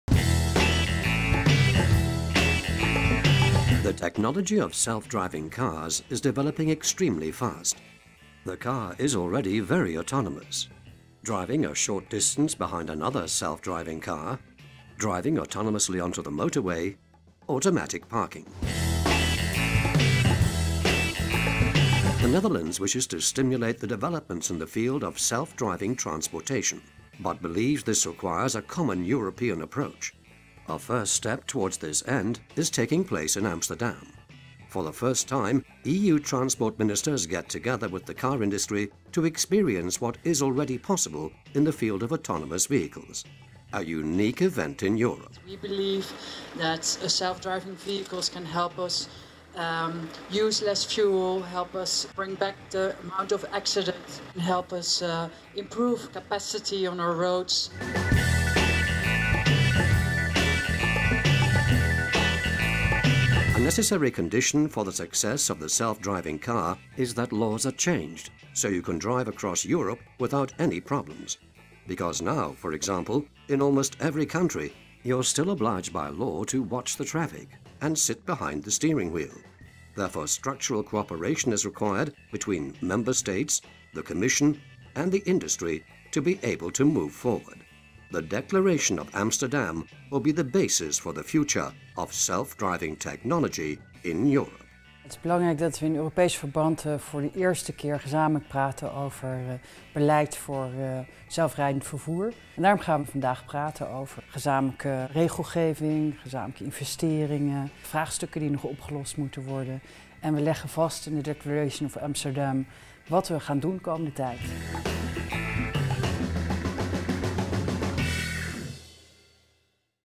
Self driving cars - EU2016NL
The idea of self-driving cars might sound futuristic to some, but on 14 April European transport ministers had put their faith in this technology and took a ride through the streets of Amsterdam. By signing the Amsterdam Declaration, a new technology agreement, Dutch transport minister Melanie Schultz will underscore the importance of effective cooperation between government and industry.
14 April 2016, Amsterdam.